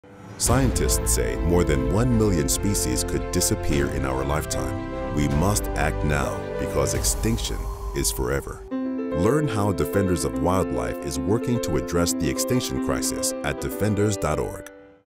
Extinction is Forever
Scientists say more than one million species could disappear in our lifetime.
We must act now because extinction is forever.
Learn how Defenders of Wildlife is working to address the extinction crisis